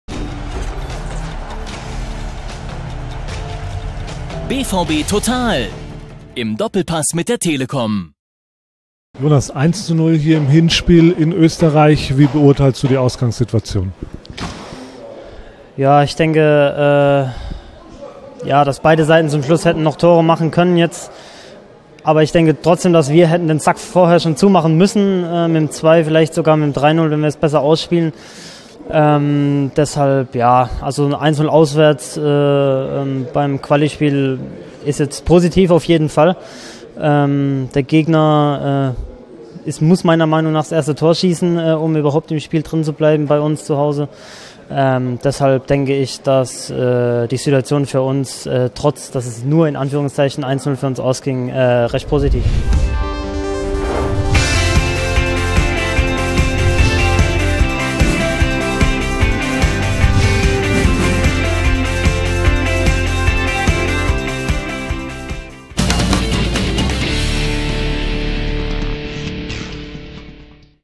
Sieg in Wolfsberg: Torschütze Jonas Hofmann im Interview
Ein Ausschnitt aus dem Interview mit Jonas Hofmann. Das komplette Gespräch mit unserem Torschützen seht Ihr im Premiumbereich